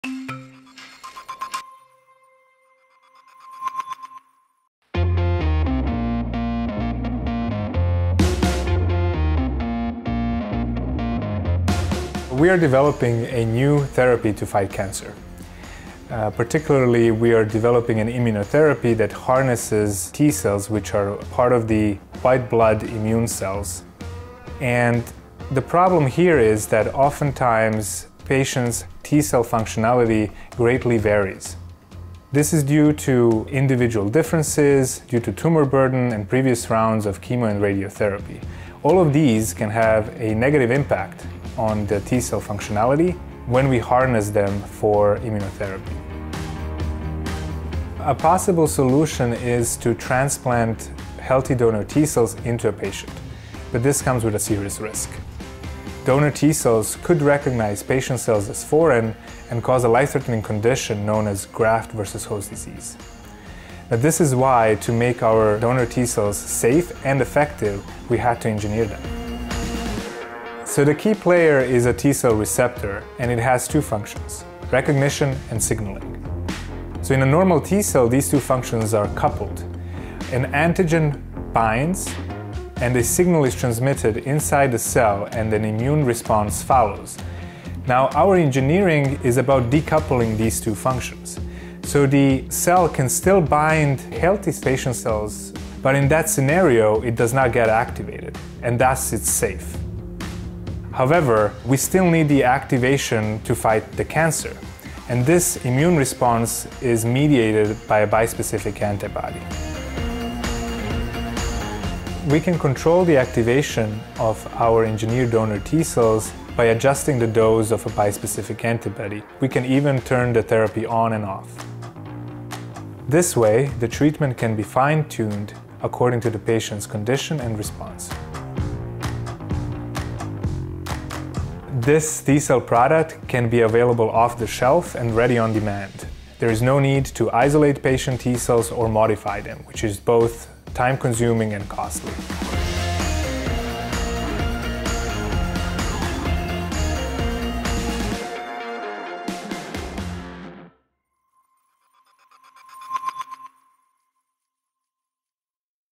Spark Award 2023 - Enhancing bispecific antibody therapies with healthy donor T cells
The Spark Award stands for the most promising invention, which ETH Zurich filed for a patent in the previous year. The presented technology is one of the top 5 finalists.

The video features Edo Kapetanovic, Sai T. Reddy, Cédric R. Weber, and Rodrigo Vazquez-Lombardi from the department of Biosystems Science and Engineering. They engineered T cells that can be safely transferred from a healthy donor to a cancer patient for immunotherapy.